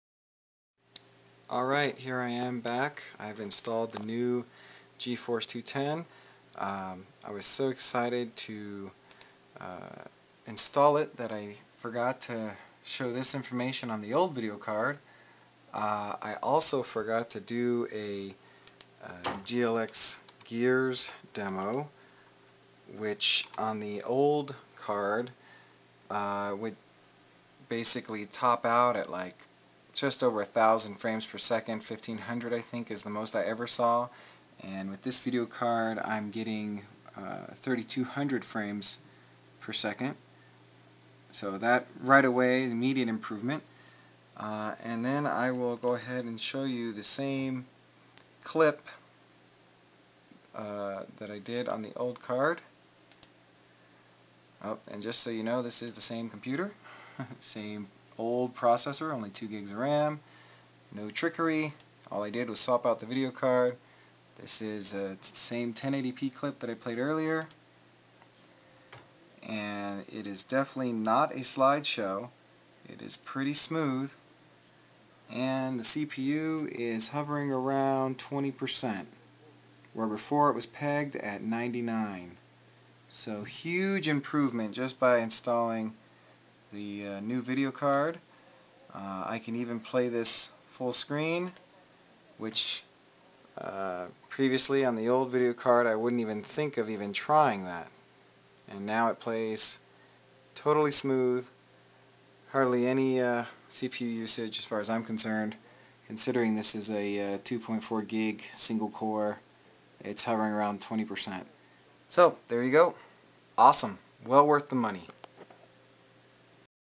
Nvidia Hardware acceleration demo on Linux (VDPAU) - Part 2
This is part two of a video where I finally got around to replacing my old Nvidia Geforce 6100 nForce 405 built in video card with a PCI Express Nvidia Geforce 210 card that supports VDPAU (hardware acceleration on Linux).